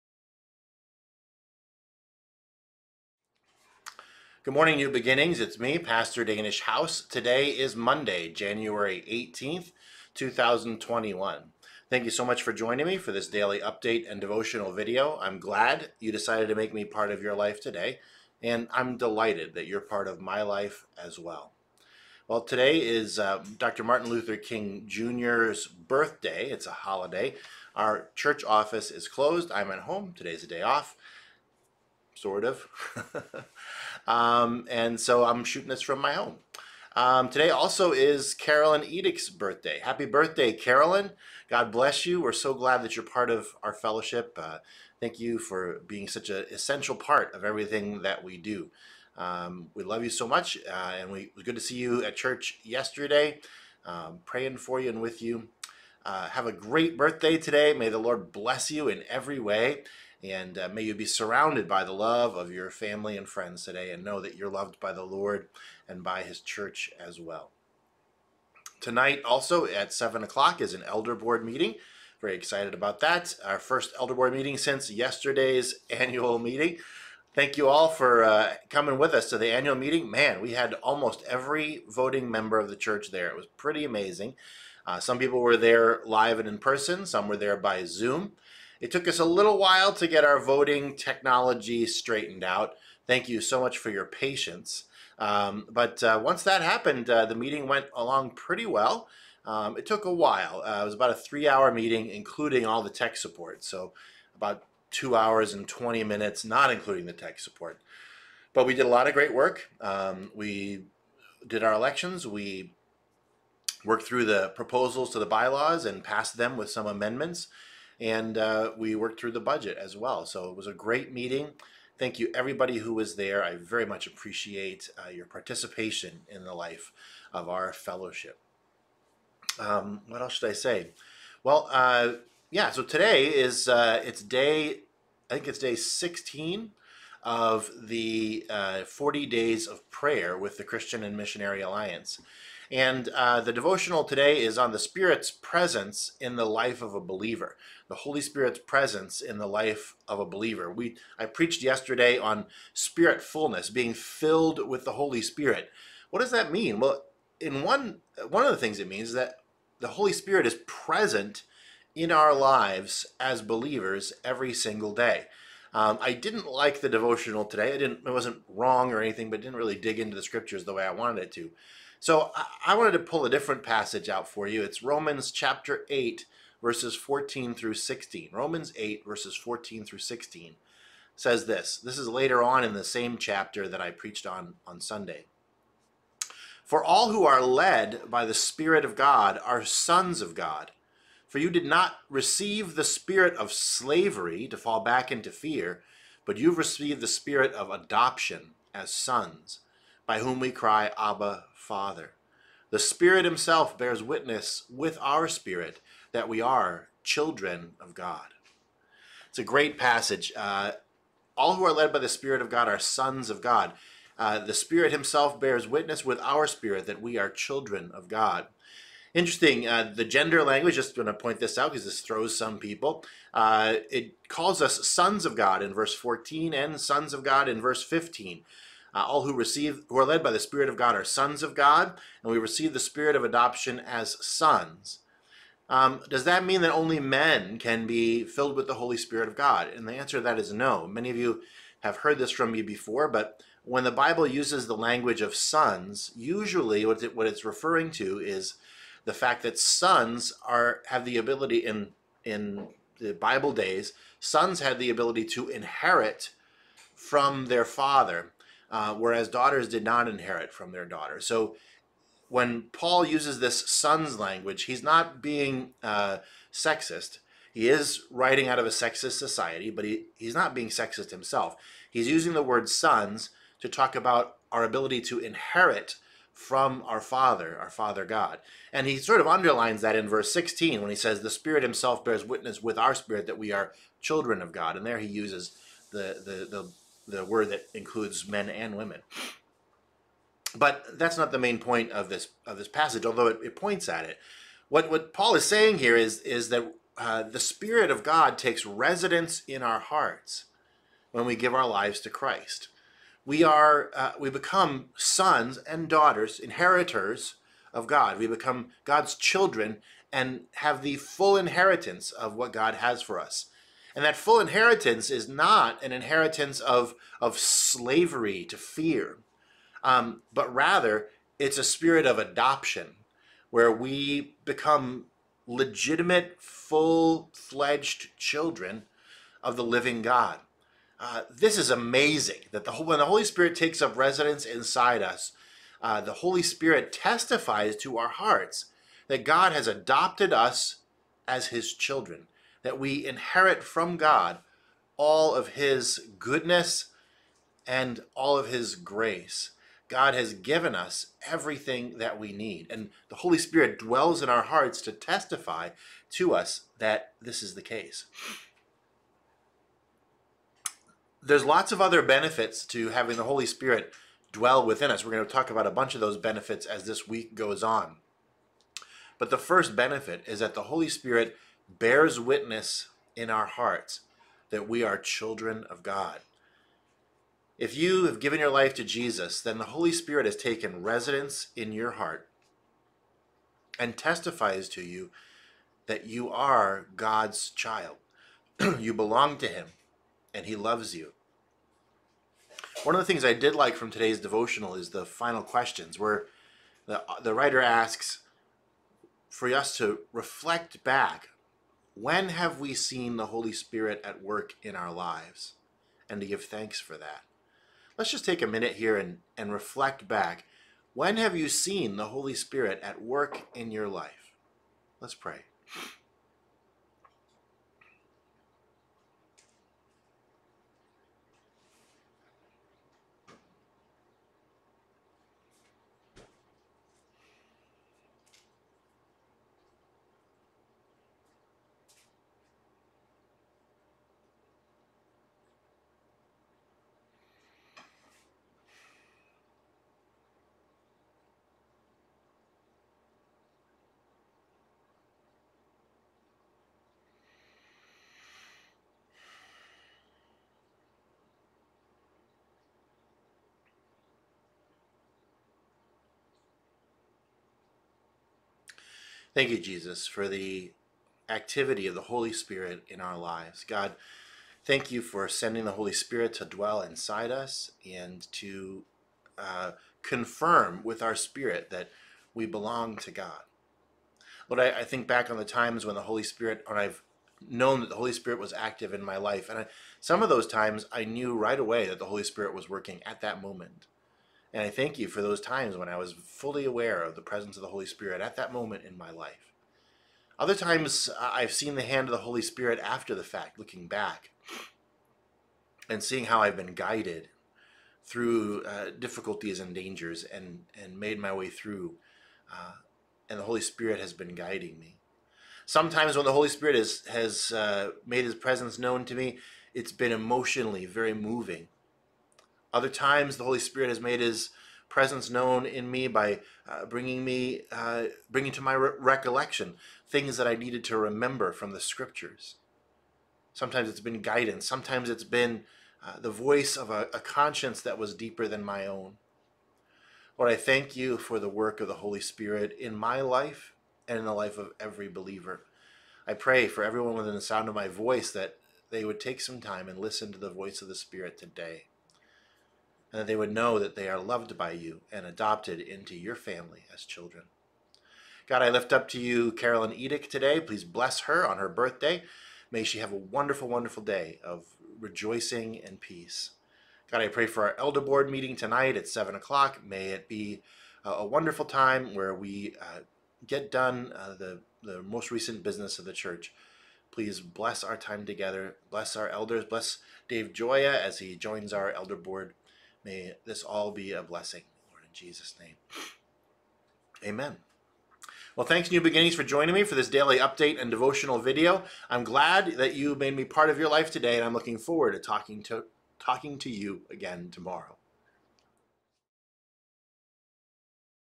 January 18, 2021 Daily Update and Devotional
What are the benefits of the Holy Spirit taking residence in our hearts, when we by faith trust Jesus? The first benefit is that we receive the spirit of adoption!

PLUS updates and prayer!